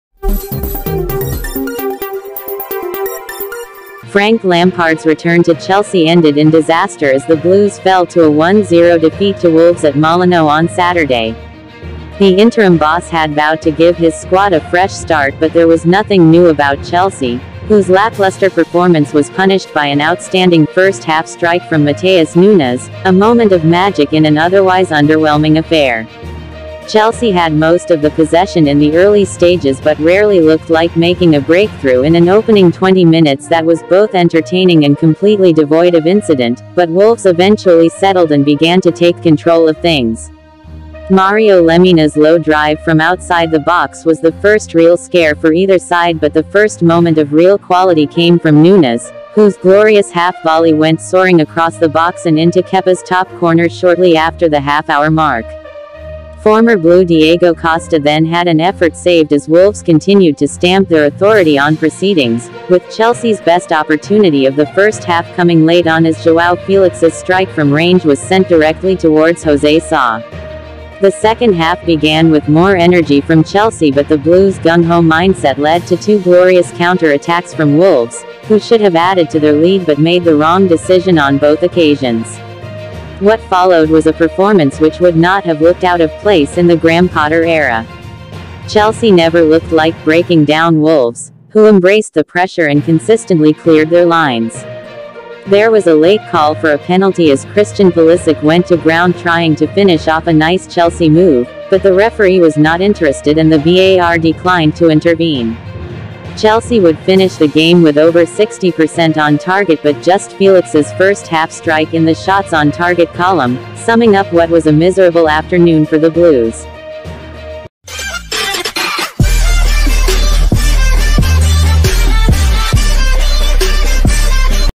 Wolves 1-0 Chelsea: Player ratings as Nunes screamer spoils Lampard's return
Wolves 1-0 Chelsea: Blues go 4 games without a win, Matheus Nunes scores

Game is over and the final score is Wolves 1-0 Chelsea. Here is the match report.

2022/2023 Premier League

1 - 0

Matheus Nunes (31')

Match Events

0' The match is about to start!

18' CHANCE! Sterling's shot blocked

22' Joao Gomes goes down in the box but ref says no penalty!

31' GOAL! Wolves 1-0 Chelsea (Matheus Nunes)

37' MISS! Diego Costa's shot denied by Kepa

39' Yellow Card! Lemina booked

49' Yellow Card! Marc Cucurella booked

79' Injured Fofana replaced by Trevoh Chalobah

85' MISS! Aubameyang's shot blocked

Line-ups

Wolves XI: Diego Costa, Dawson, Lemina, Semedo, Jose Sa, Podence, Kilman, Matheus Cunha, Matheus Nunes, Toti Gomes, João Gomes

Subs: Moutinho, Sarabia, Daniel Bentley, Traore, Hwang Hee-Chan, Pedro Neto, Joe Hodge, Nathan Collins, Hugo Bueno

Chelsea XI: Koulibaly, Kovacic, Sterling, Kepa, Marc Cucurella, Havertz, Reece James, Felix, Conor Gallagher, Wesley Fofana, Enzo Fernández

Subs: Aubameyang, Edouard Mendy, Loftus-Cheek, Trevoh Chalobah, Chilwell, Zakaria, Pulisic, Benoît Badiashile, Mykhaylo Mudryk